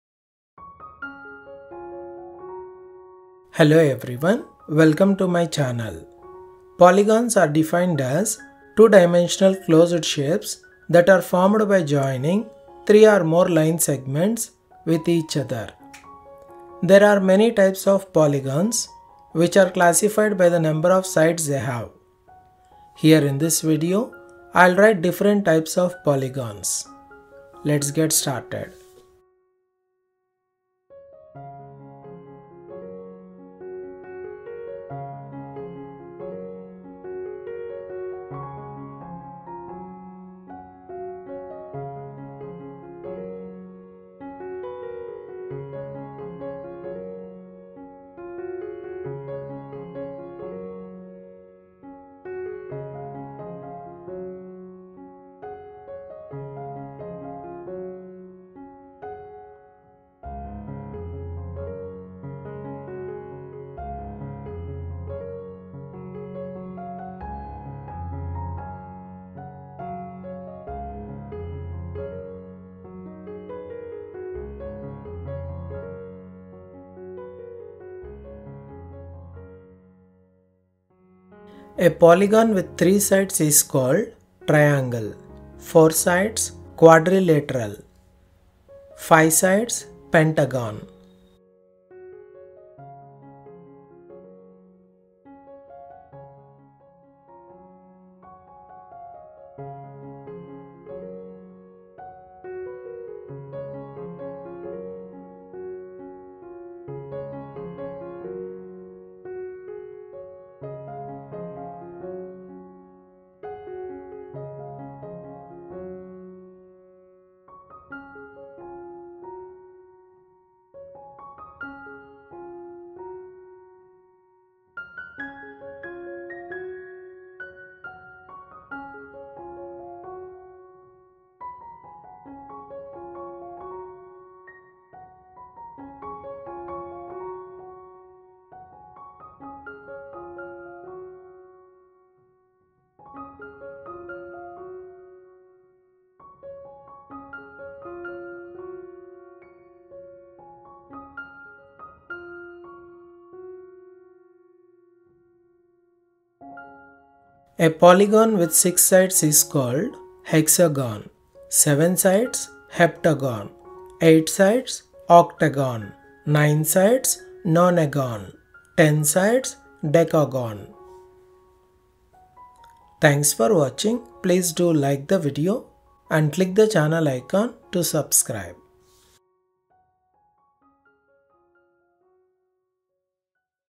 NAME OF POLYGON | TYPES OF POLYGON
Looking for a video on Polygon? Check out this video with the names of polygons, which explains the topic easily.

name of polygon
types of polygon